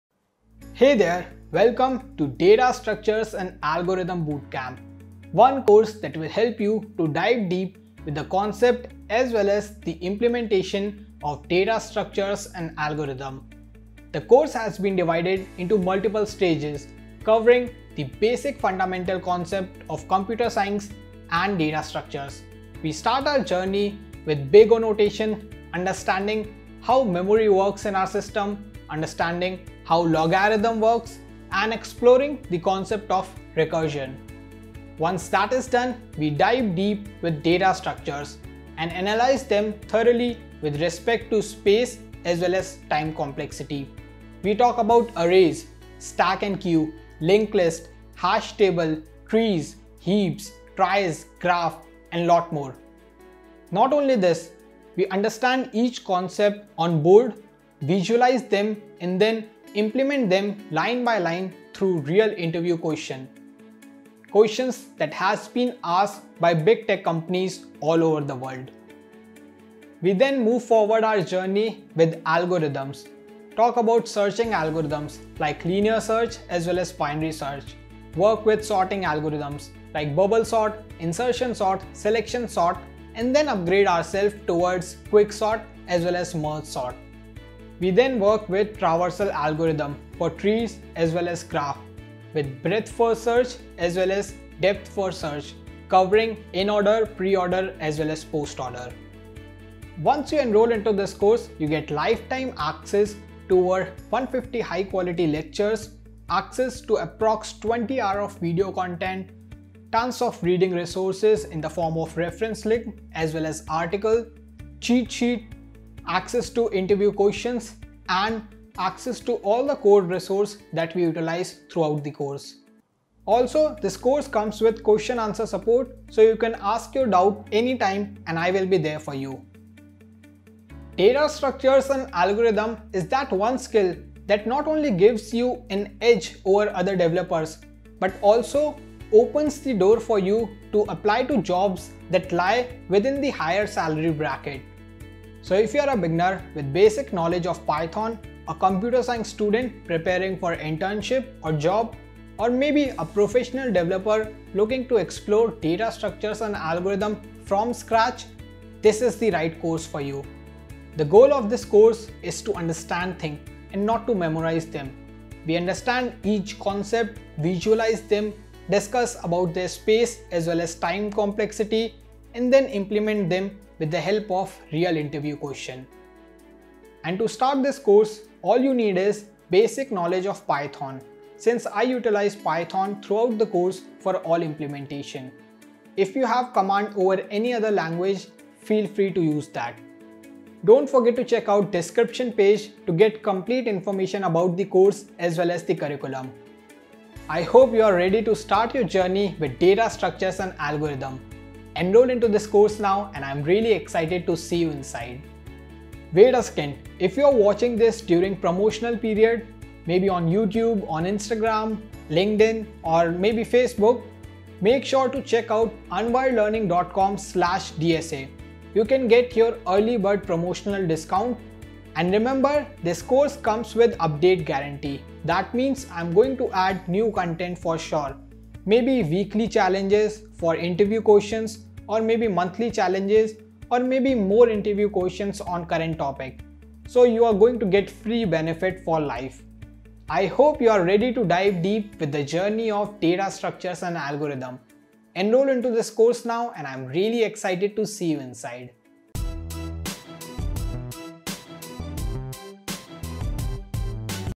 DSA Course - Data Structures and Algorithms Complete Bootcamp
Complete data structures and algorithms training for beginners. Start DSA from basics to advanced by understanding concepts, visualizing them, and then implementing through Python with Interview Questions.

The course is recommended for beginners who want to start data structures and algorithms from basics.